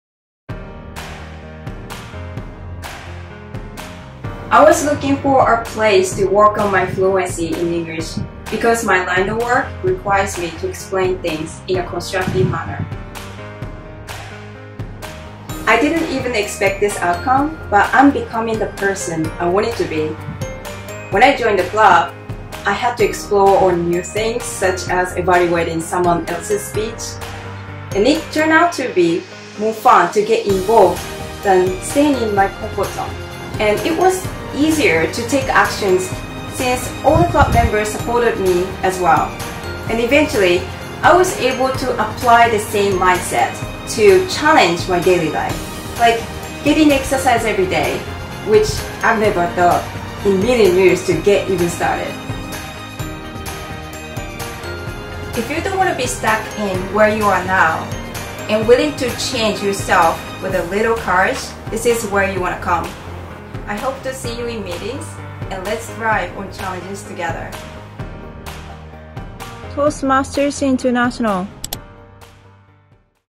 トーストマスターズ ストーリーズ「I could change my mindset」西川郁美さん
「トーストマスターズ ストーリーズ」はトーストマスターズ インターナショナルに在籍する会員が①なぜトーストマスターズに入ろうと思ったのか　②どのような変化があったのか　③どんな人にお勧めしたいのか　を簡潔にまとめた動画シリーズです。

英語が上達したい、人前で上手く話せるようになりたい、モチベーションの高い仲間が欲しい、など理由は様々ですが、一人一人に違ったストーリーがあります。

この動画を見て興味を持たれた方は、ぜひ一度お近くのクラブに見学に行ってみて下さい。新しい世界が広がるかも知れません。